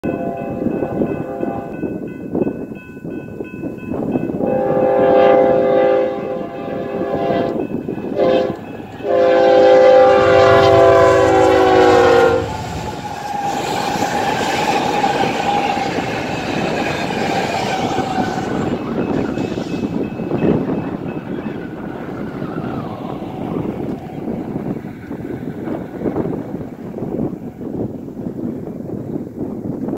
L😳😳K!! BNSF 8-Units Screaming Solo at 70-mph! Must See!! 😎
Eastbound at Verona, IL.